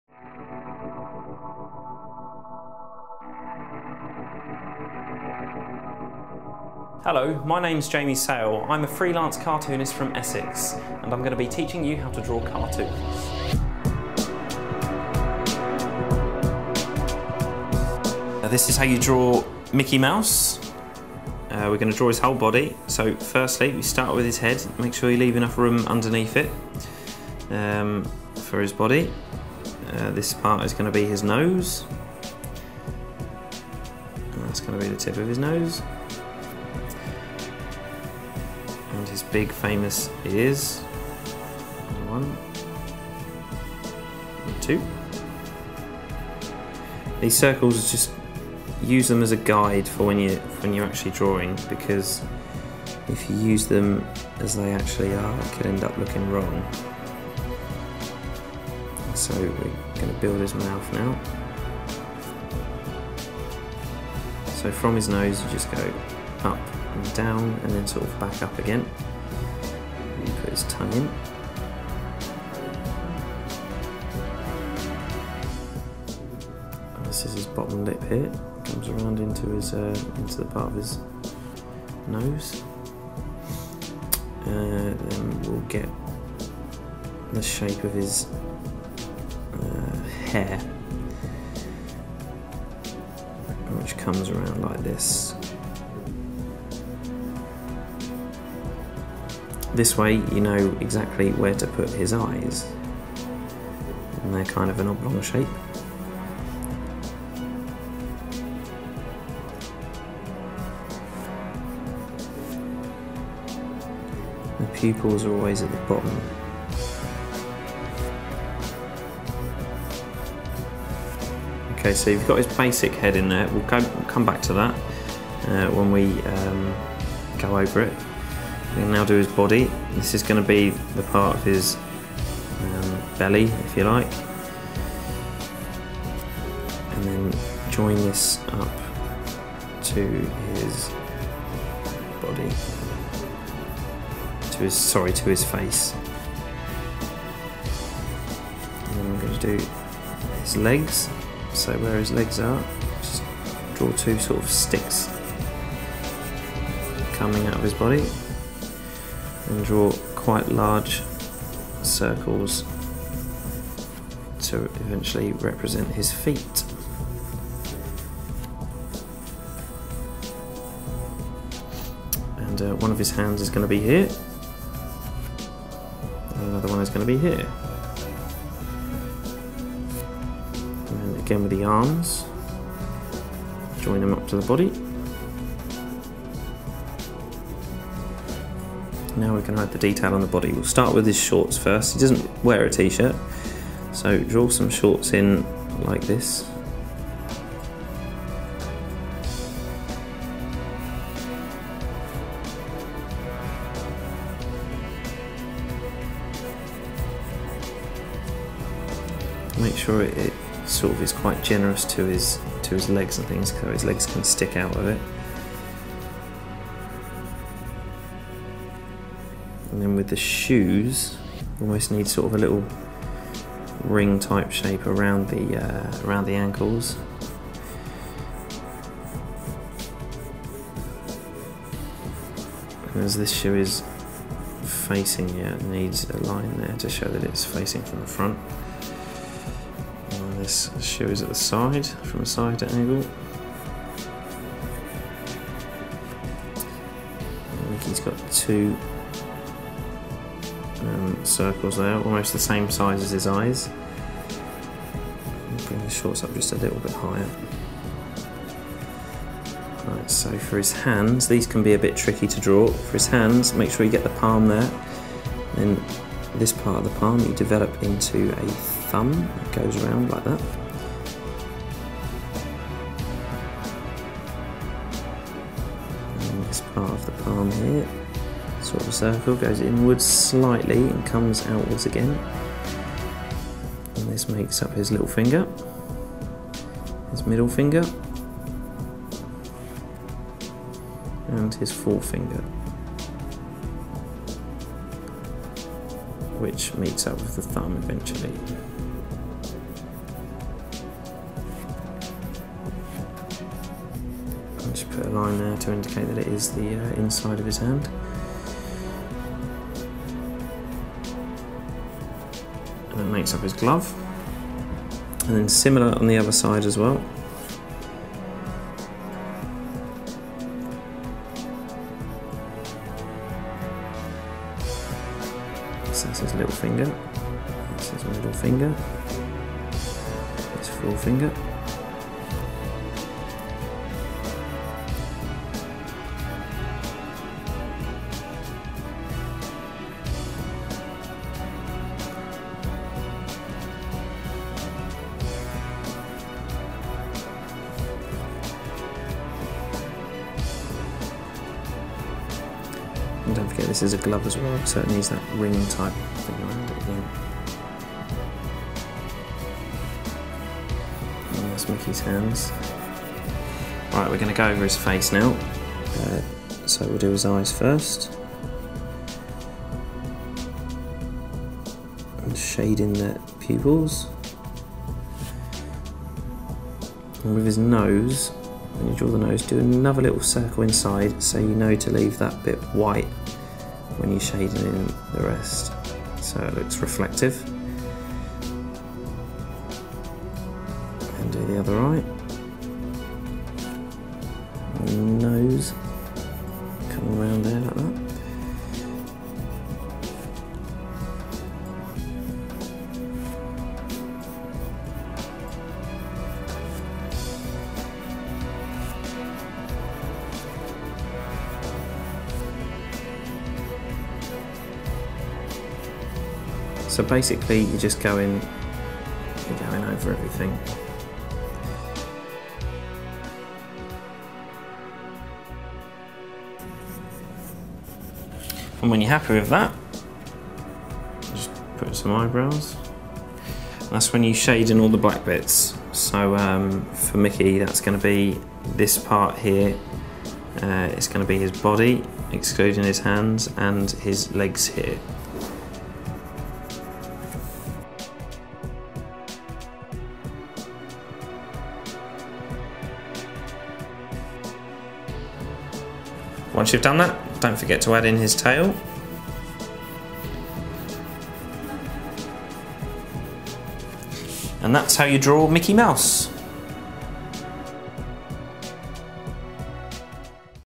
How To Create A Mickey Mouse Drawing
Watch How To Create A Mickey Mouse Drawing from the pioneers of how to videos. This instructional video will give you step-by-step instructions to ensure you get good at cartoon drawing.